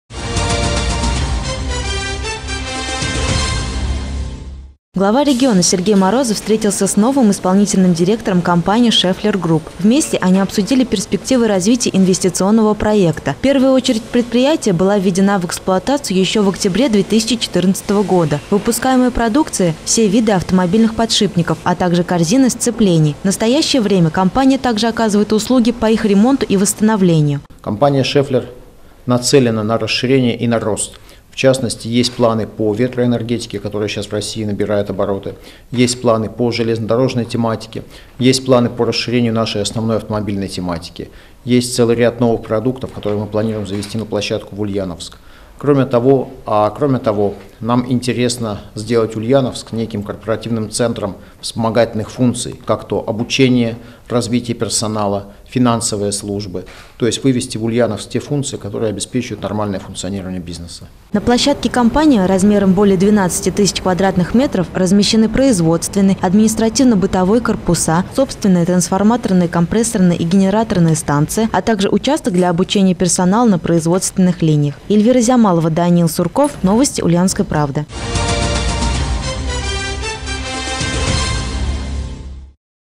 Компания «Шэффлер» расширит производство в Ульяновской области
Глава региона Сергей Морозов встретился с новым исполнительным директором компании Максимом Шаховым.